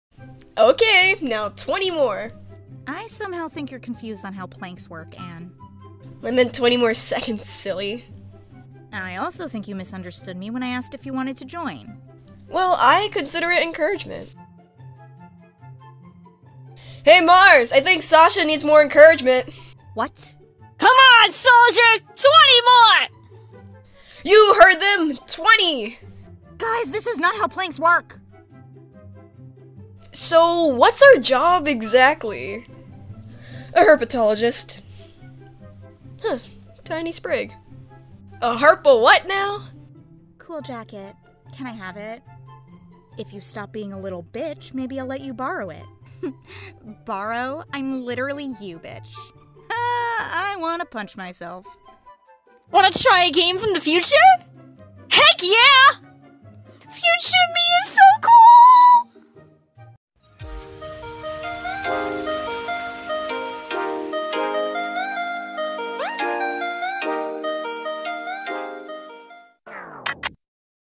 Sasha's Uniquely Hilarious Plank Workout ( Amphibia Comic Dub )
In Today's Amphibia Comic Dub, Sasha has a Unique and Hilarious Plank Workout session and then The Calamity Trio Meet up with their future selves in this Hilarious Comic Dub Compilation

🎙️People Involved 🎙️

🎨 Comic Featured 🎨:

(0:00)Sasha's Uniquely Hilarious Plank Workout is by Galactic_Flows_Draws on Instagram:

(0:29)The Calamity Trio Meet Their Future Selves is by Amai_Matcha